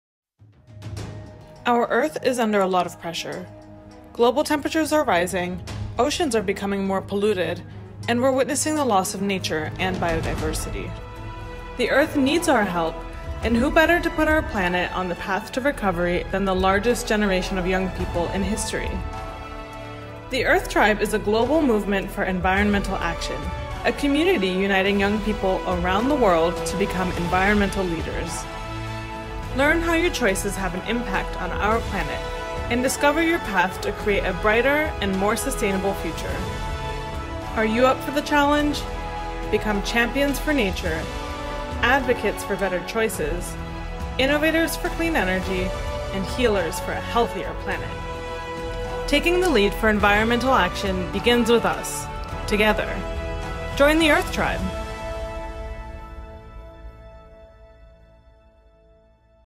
Earth Tribe by Scouts
Tο Earth Tribe είναι η νέα εκπαιδευτική παγκόσμια πρωτοβουλία των Προσκόπων.
Μέσω του Earth Tribe οι υπεύθυνοι και ενεργοί πολίτες της παγκόσμιας κοινότητας θα προσπαθήσουν να προστατέψουν ακόμη περισσότερο τον πλανήτη μας.